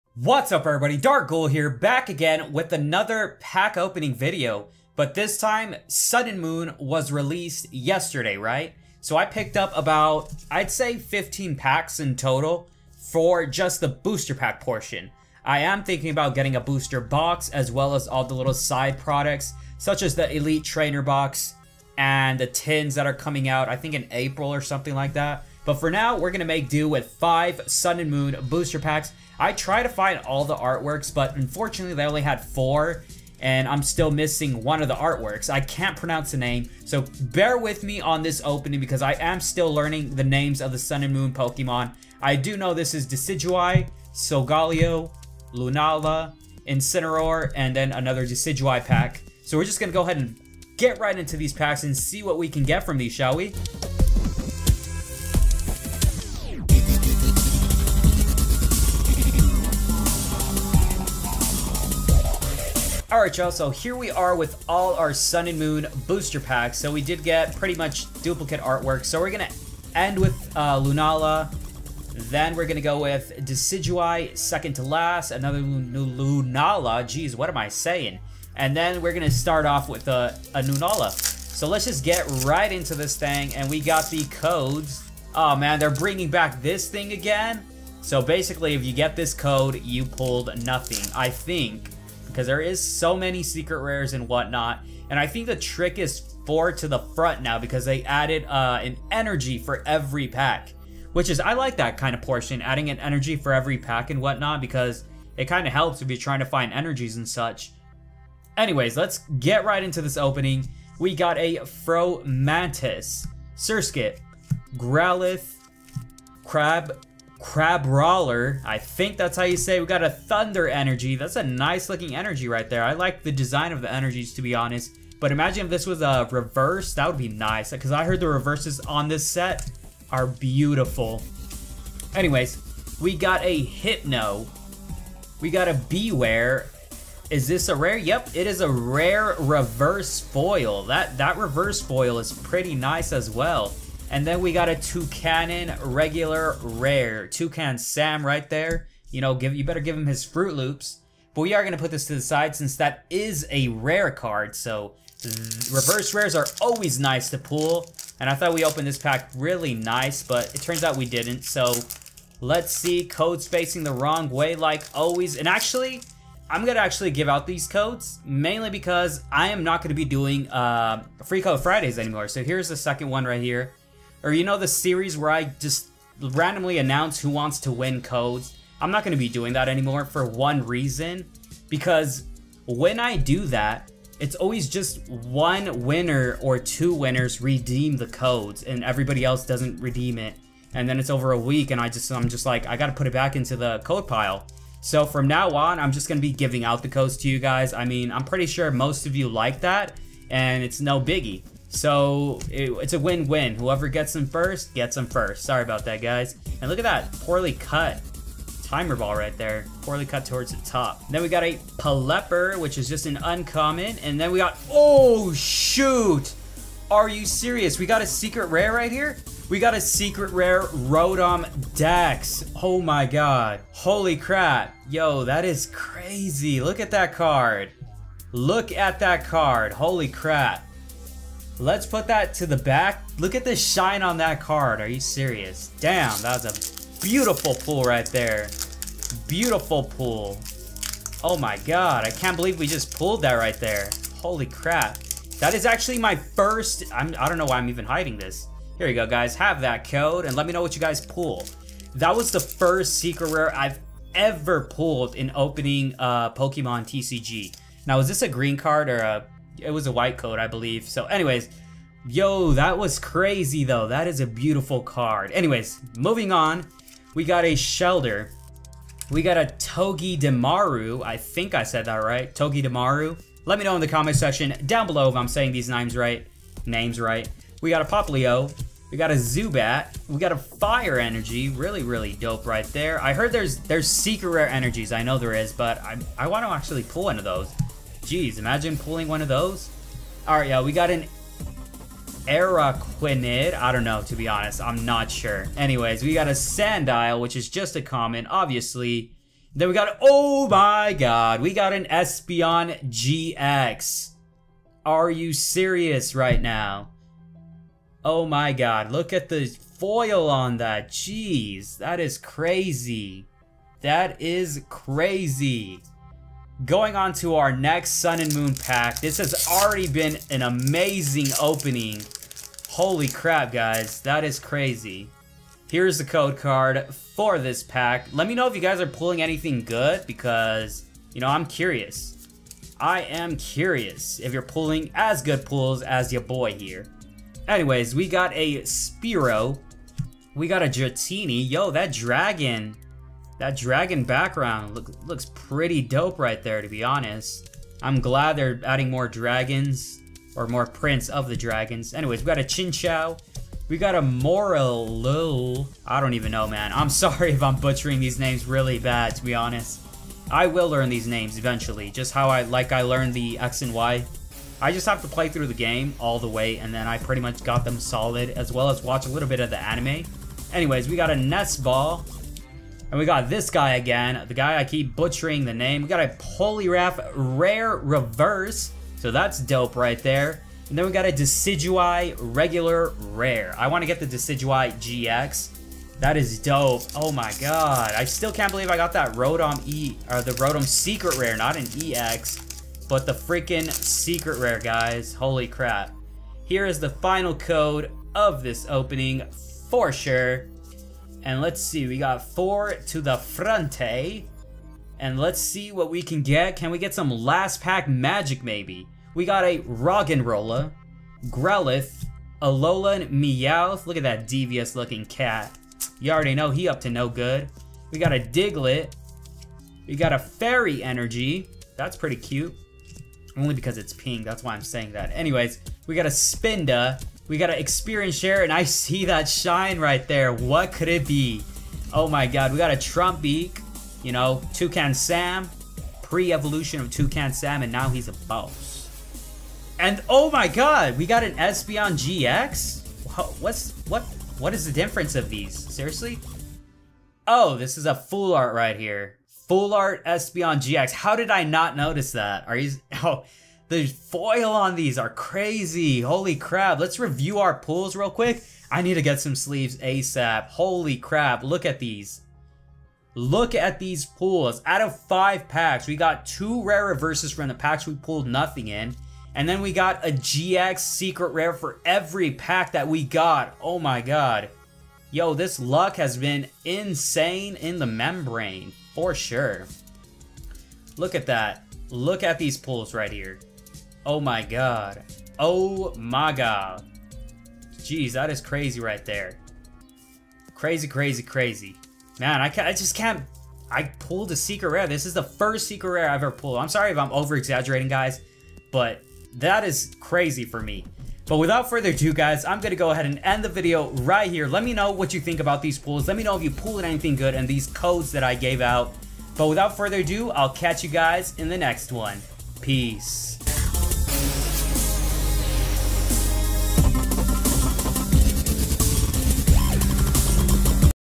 OPENING THE BEST 5 POKEMON SUN AND MOON PACKS EVER!
Today we open up 5 pokemon sun and moon booster packs, and well you be the judge for the pulls bc they were AMAZING!

Few updates coming soon!